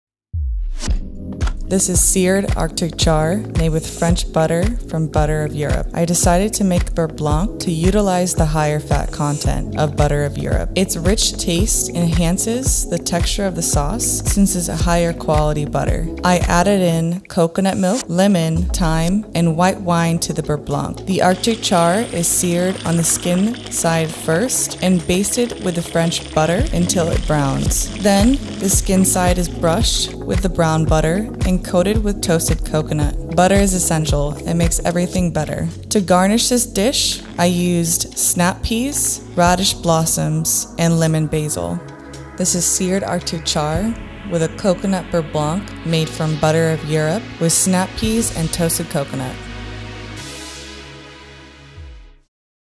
Test Kitchen: Arctic Char Coconut Beurre Blanc
We invited Chef Moira Hill @redshortchef to the crTestKitchen where she made a pan-seared coconut crusted arctic char and a coconut beurre blanc using the flavorful, rich French butter from Butter of Europe✨